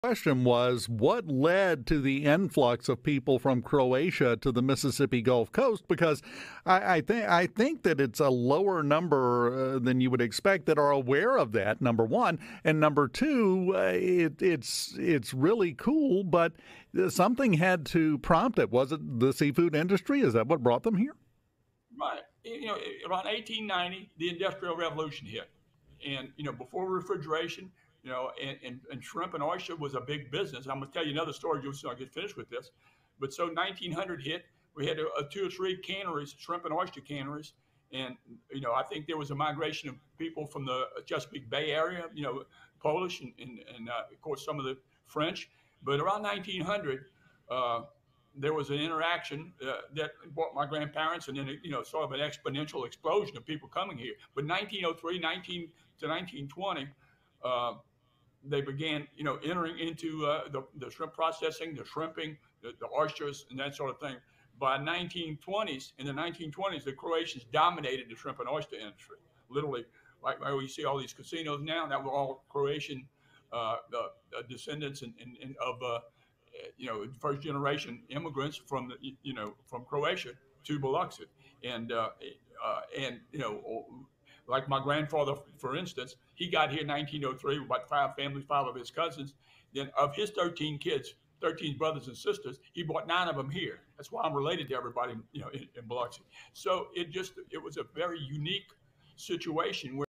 The MS Gulf Coast's Croatian History and Heritage
Andrew "Fofo" Gillich(Mayor of Biloxi) discusses the Croatian President visiting Biloxi tomorrow, and the Mayor breaks down some of the Croatian heritage and history on the Mississippi Gulf Coast.

To see the rest of the conversation, head on over to our Website to watch the entire conversation On Demand!